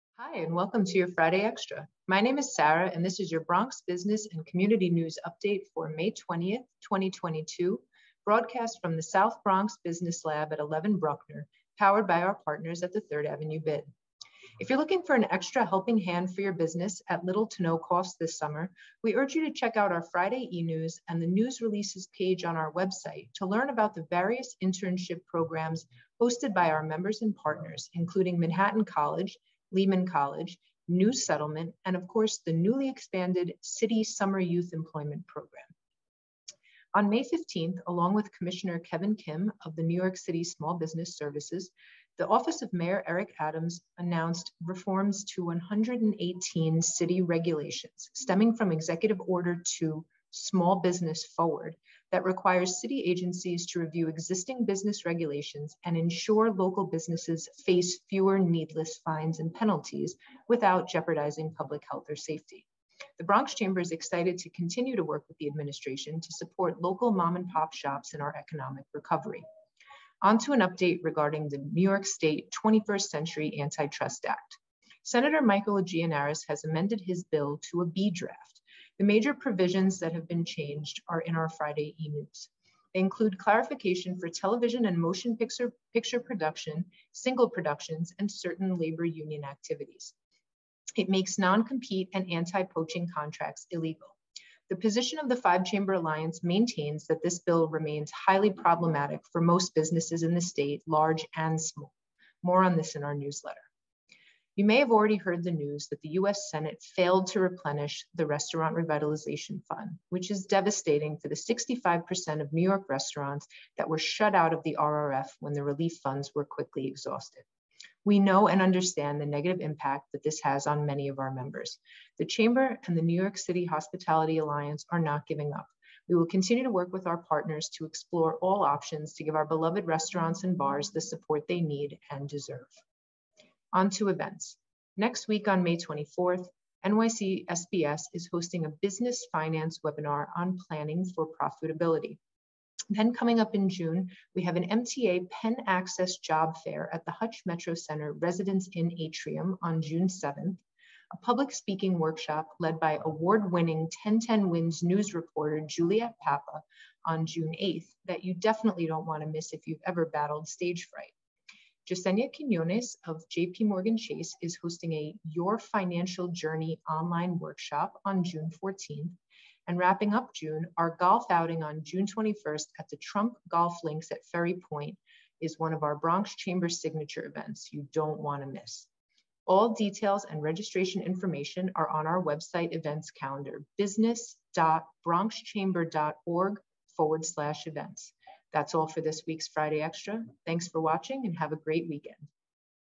May 20, 2022 Friday Extra
This week's Friday Extra covers the 21st Century Antitrust Act Update,
get a helping hand & provide opportunities for Bronx youth with available internship programs, update on the Restaurant Revitalization Fund,
Mayor Adams announces 118 city regulation reforms, and
upcoming events.